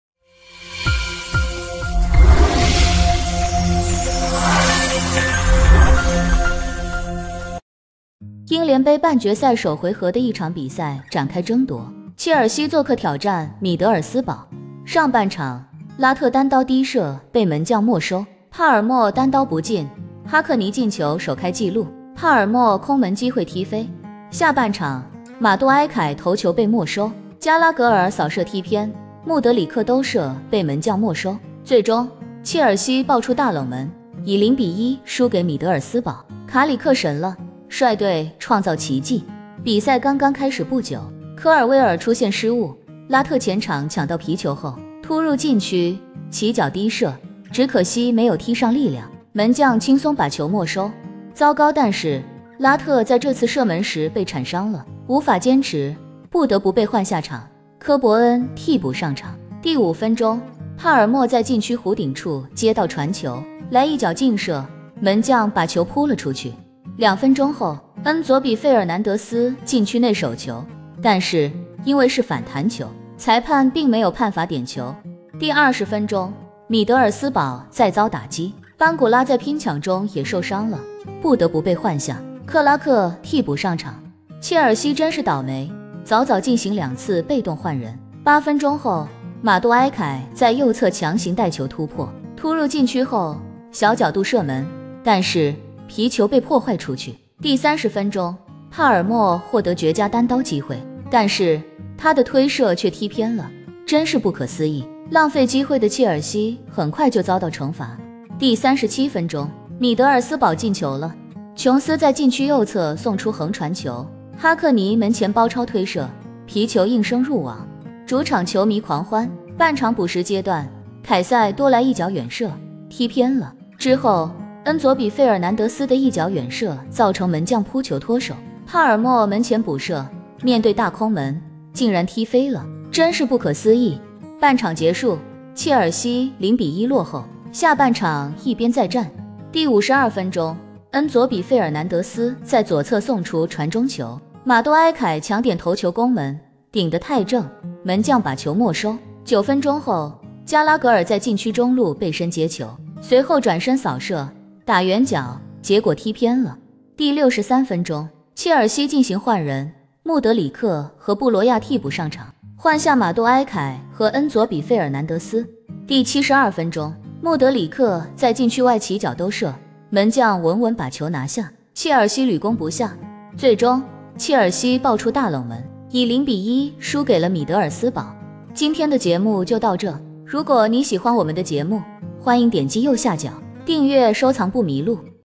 冷！0-1! 切爾西 倒下了，卡里克是神，是奇跡，帕爾默的空門沒進！
英聯杯半決賽首回合的一場比賽展開爭奪，切爾西做客挑戰米德爾斯堡。上半場，拉特單刀低射被門將沒收，帕爾默單刀不進，哈克尼進球首開紀錄，帕爾默空門機會踢飛。下半場，馬杜埃凱頭球被沒收，加拉格爾掃射踢偏，穆德里克兜射被門將沒收。最終，切爾西爆出大冷門，以0-1輸給米德爾斯堡。